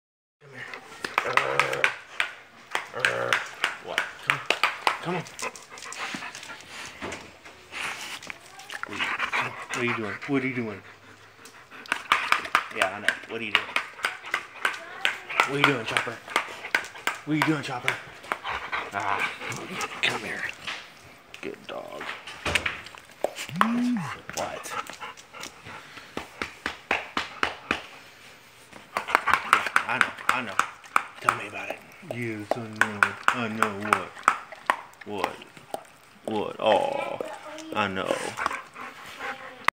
Chopper the Dog Playing
Chopper's owners are deaf, so instead of barking she has learned to communicate by "clapping" her jaws. Strangers always think she is aggressive or going to bite, but she is the sweetest dog in the world just trying to communicate.